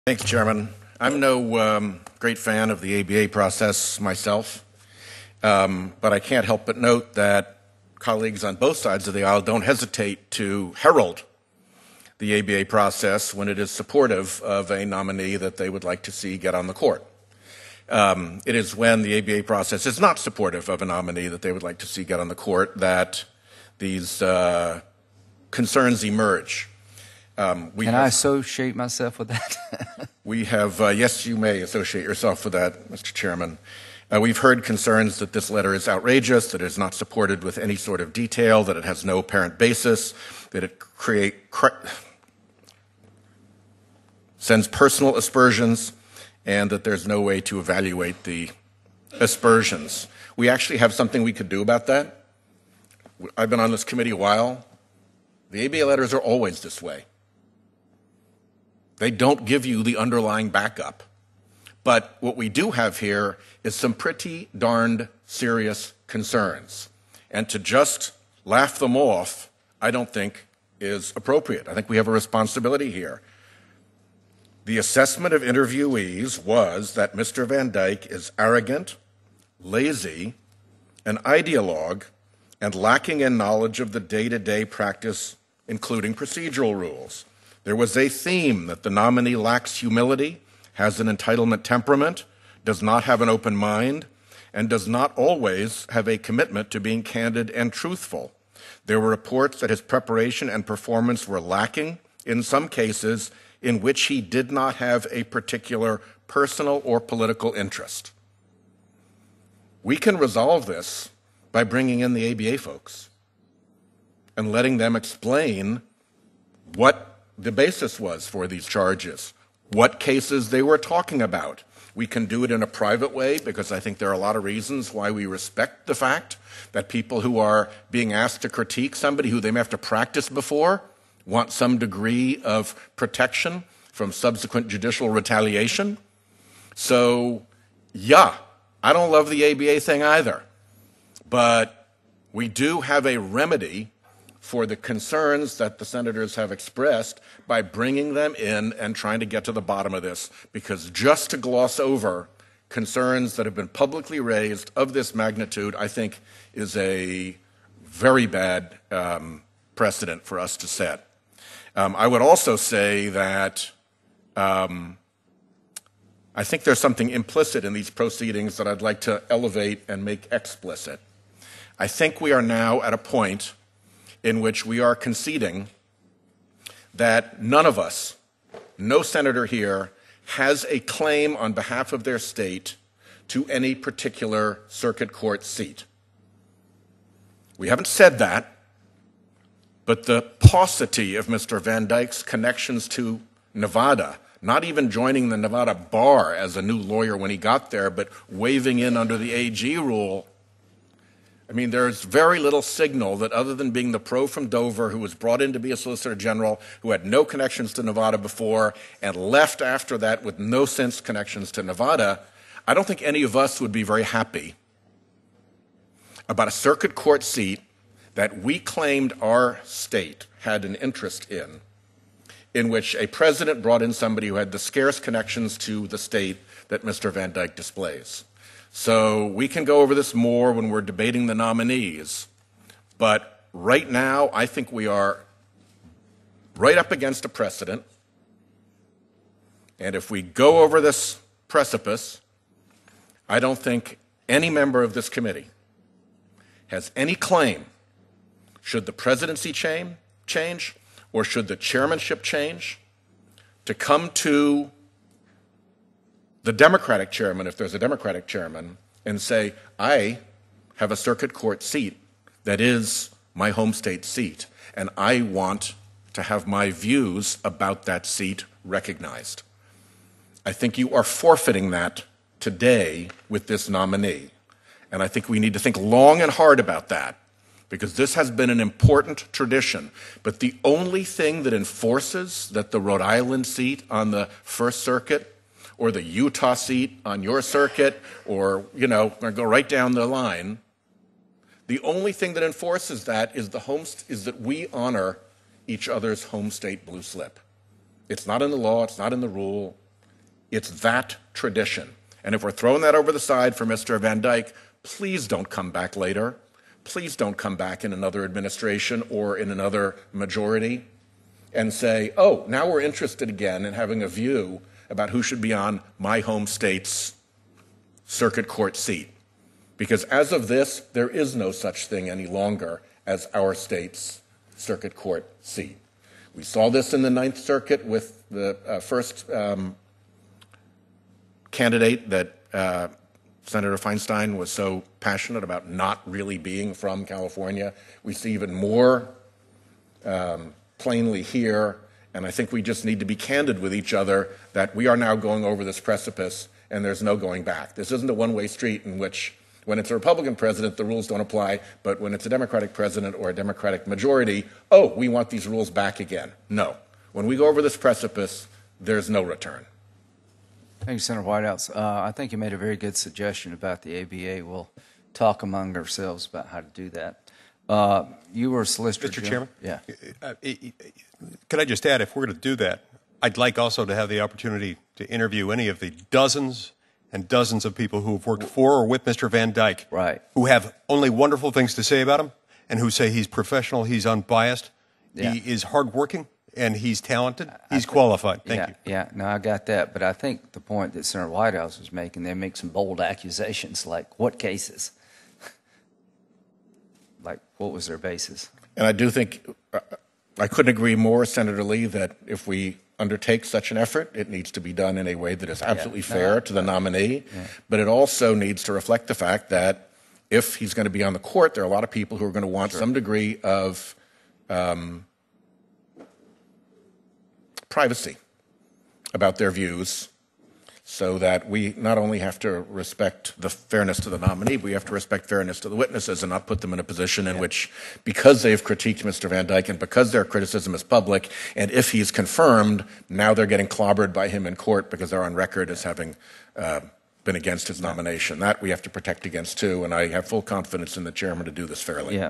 Senator Whitehouse Remarks in Senate Judiciary Nominations Hearing
On October 30, 2019, Senator Sheldon Whitehouse delivered these remarks during the Senate Judiciary Committee nominations hearing. Patrick J. Bumatay and Lawrence VanDyke are both nominated to be United States Circuit Judges for the Ninth Circuit.